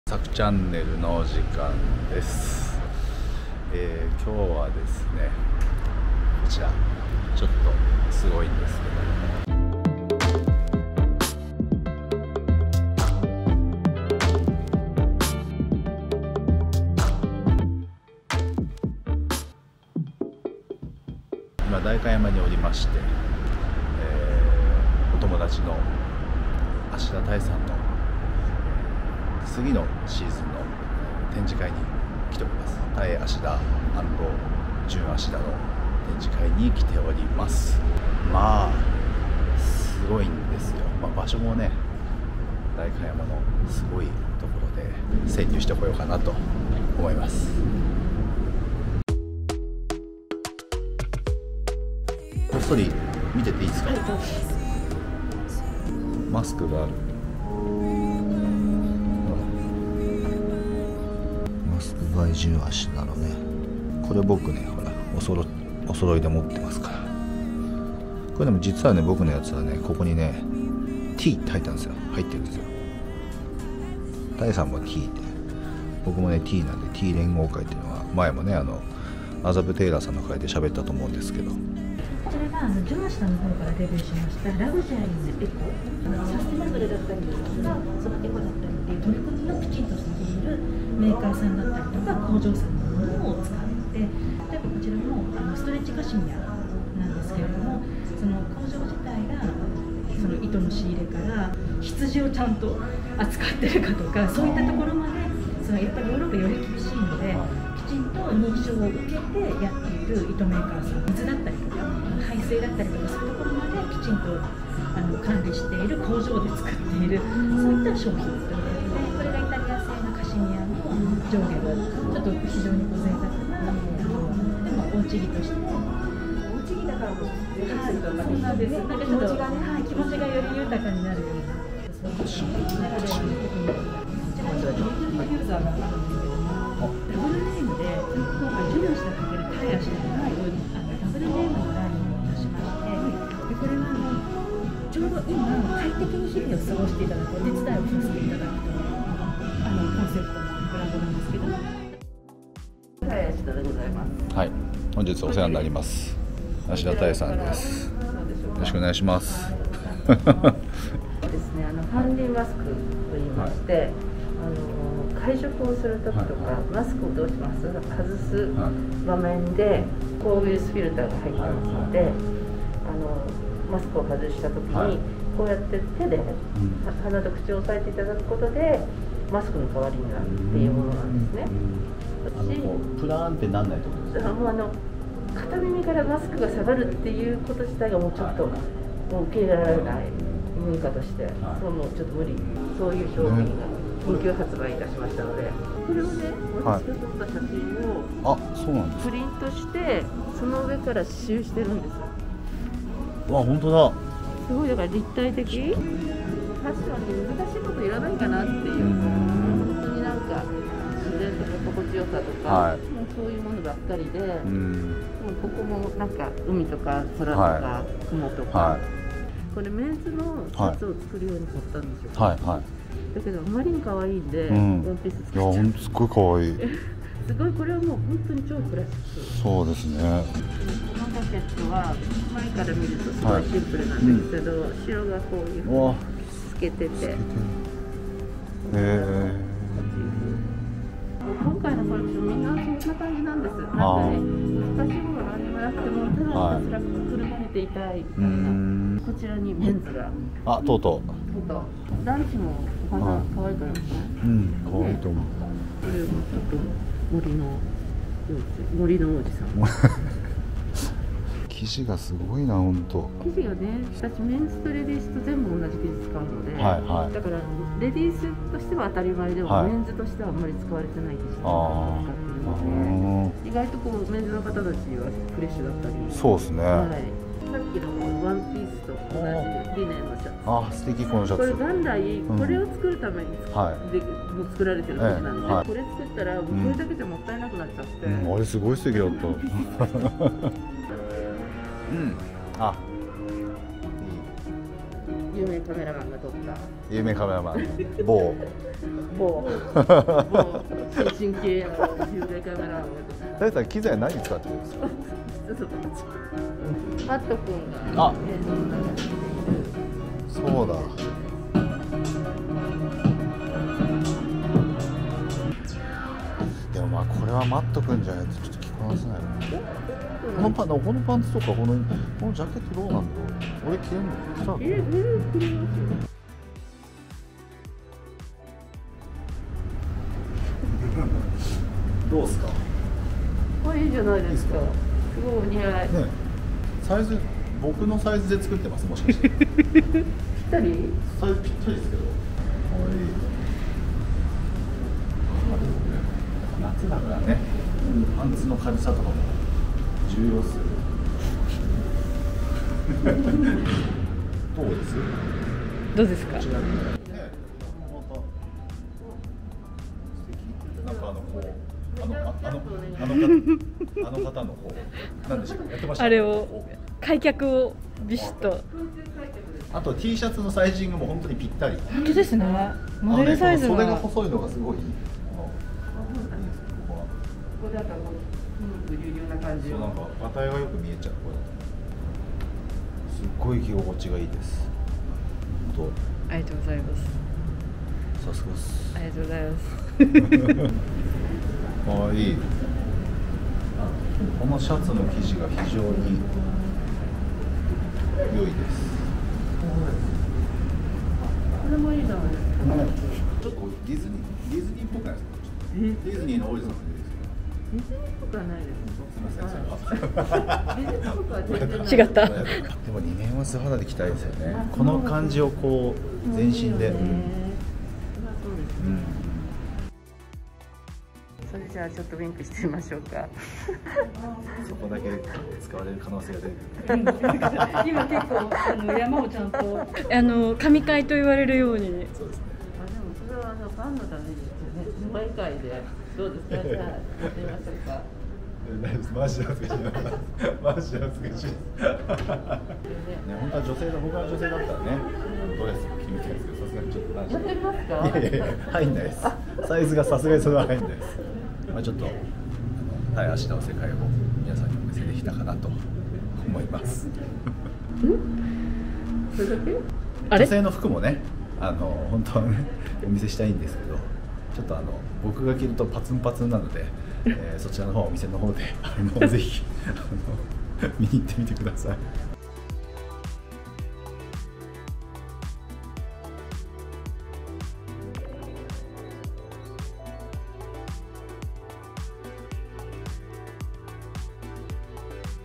jun ashida TAE ASHIDA 展示会に潜入してきました【2021SS】
仲良しの芦田多恵さんのブランド
jun ashida TAE ASHIDA 2021SS展示会に潜入して来ました
jun ashidaといえば当時の皇后陛下美智子様の御洋服を手がけられておりましたそんな由緒正しいブランドなのです
TAE ASHIDA、MASK by jun ashida、デビューブランドMes ailes等、隅々までご紹介頂きました

展示会の全貌を是非ご覧下さい！